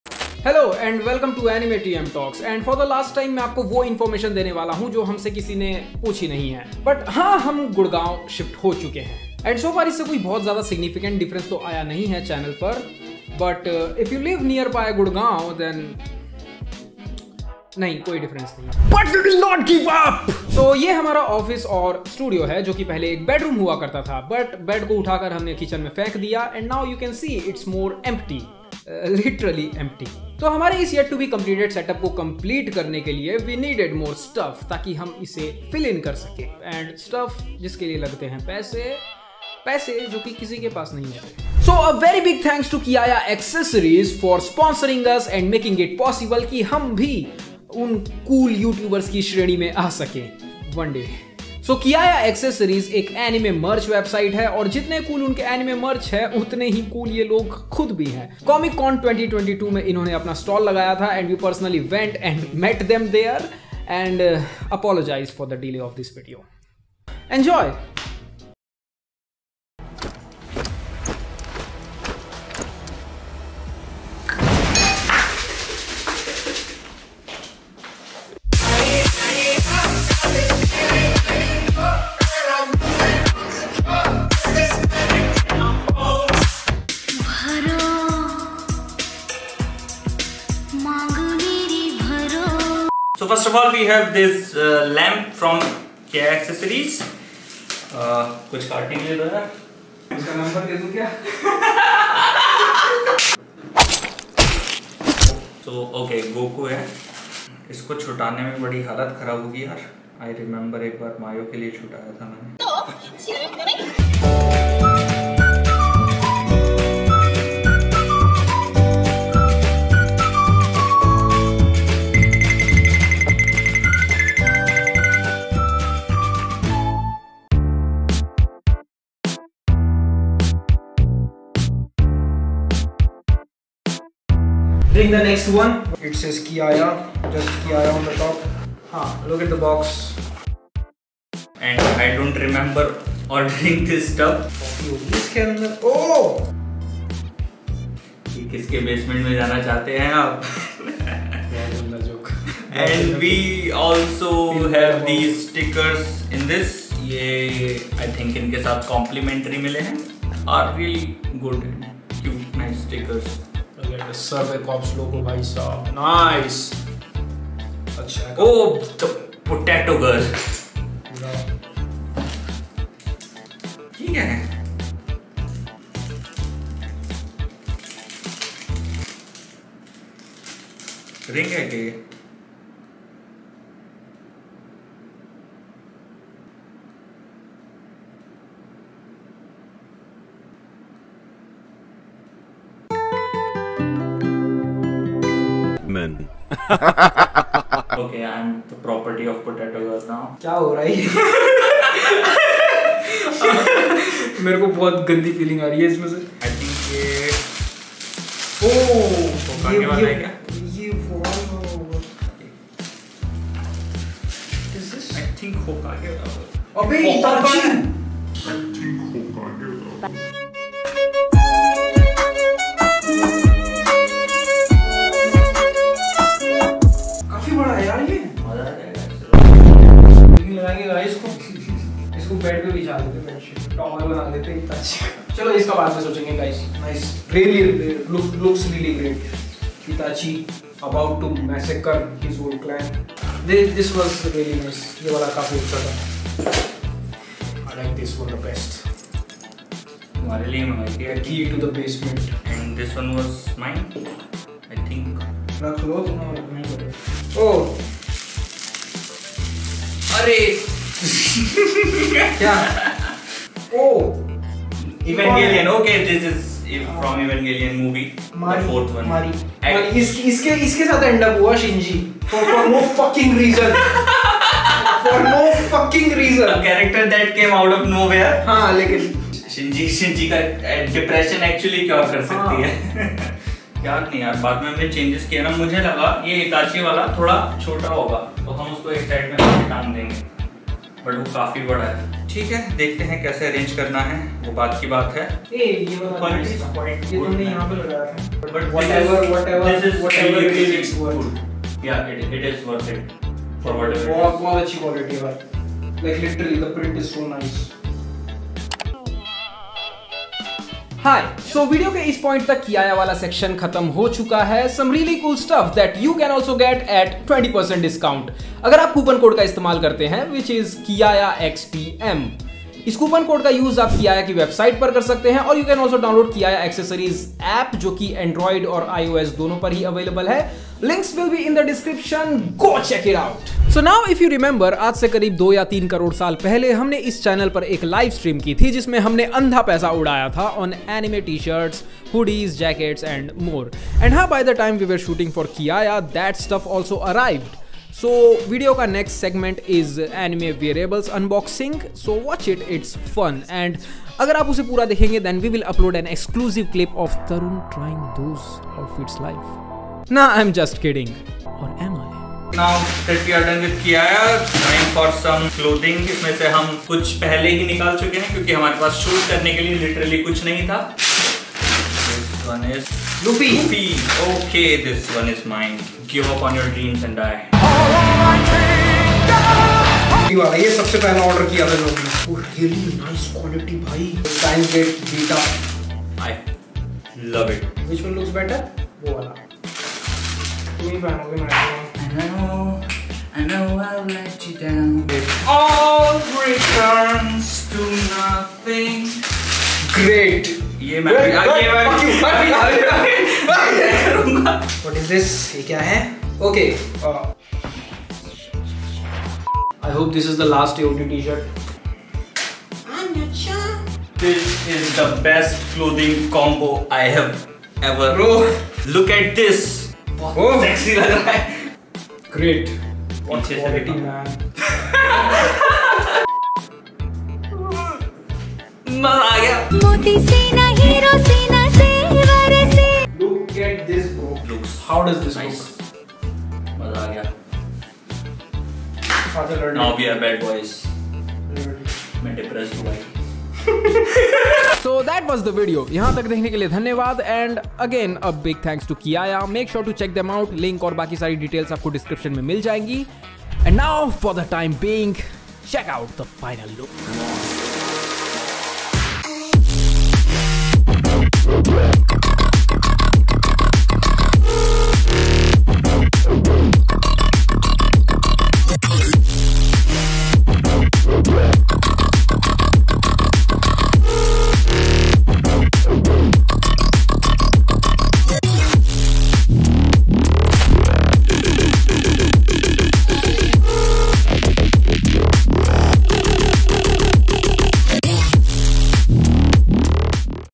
We Bought Anime Stuff For Our Weeb Setup (Lut gaye)
Two wannabe Anitubers got sent their favorite Anime merch. Thanks to Kiaya Accessories!

Kiaya Android App :- shorturl.at/v0135
Kiaya on IOS :- shorturl.at/pLY03

in this video:
0:00 - Intro
1:08 - Unboxing Some Cool Merch by Kiaya Accessories
5:33 - Showing of Our New Weeebish Tees and Drips
7:34 - Final Look of the Setup